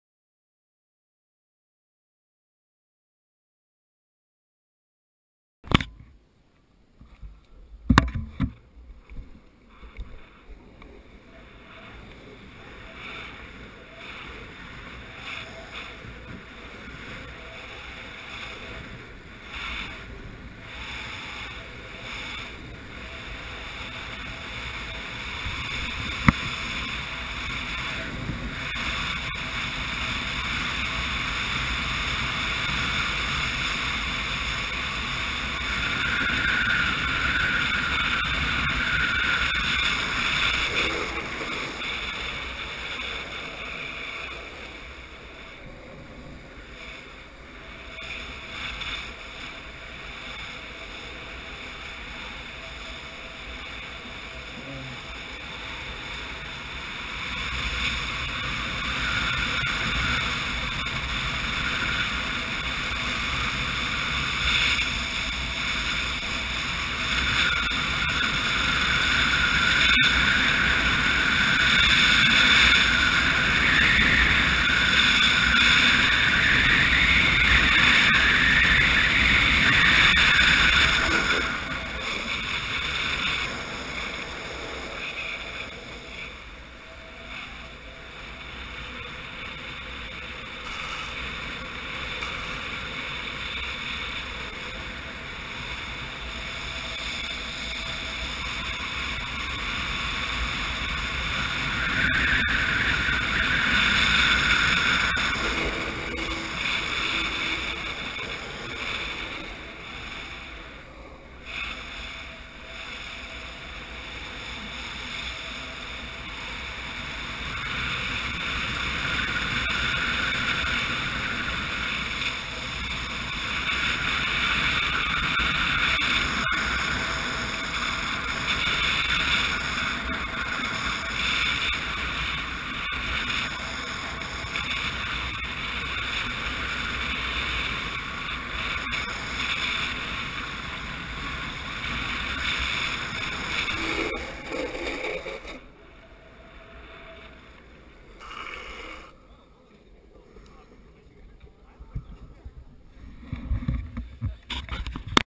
Raw run TMI Lilyhammer IDF world cup 2015
A chill warm up run at his years IDF TMI Lilyhammer worldcup in Norway. Thanks to Mills skateshop, Slipstream Longboards and Divine Wheels